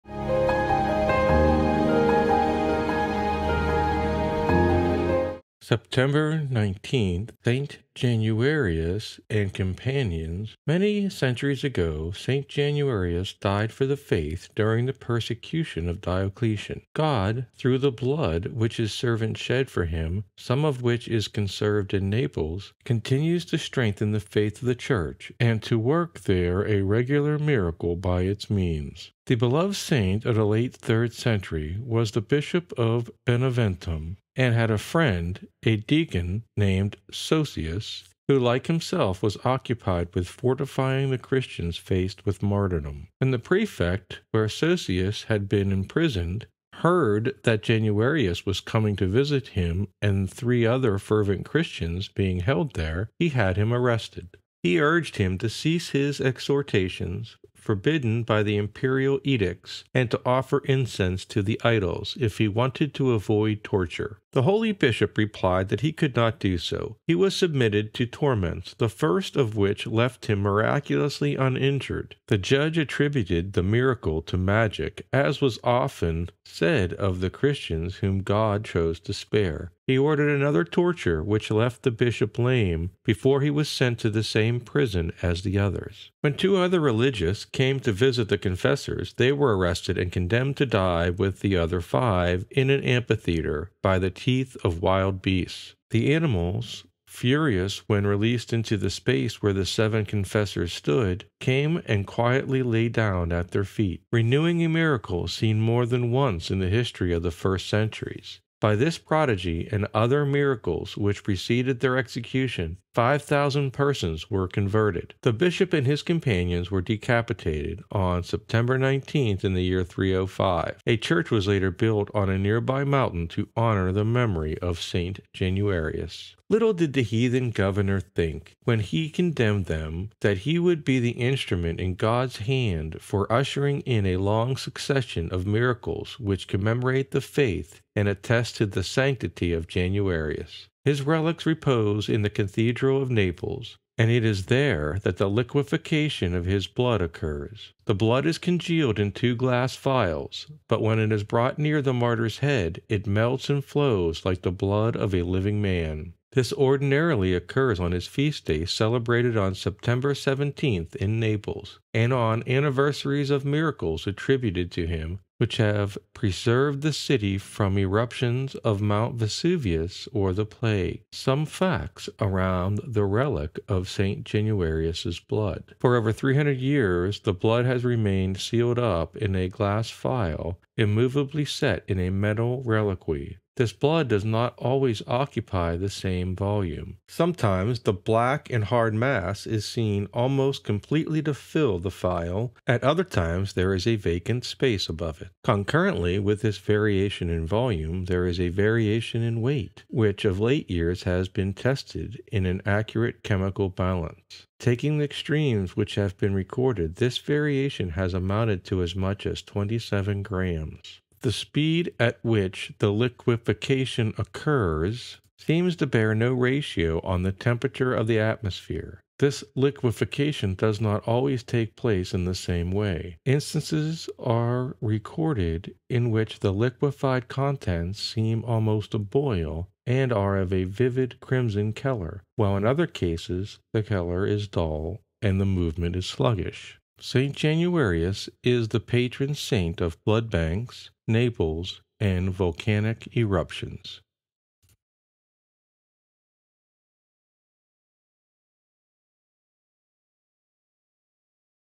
Saint Januarius and Companions, Martyrs - September 19th
Born:  3rd century
Died: September, 19, 305

Martyrdom:  first thrown to wild beasts;
                        when the animals would not
                          attack him, he was beheaded

Patronage:
 blood banks; Naples;  volcanic eruptions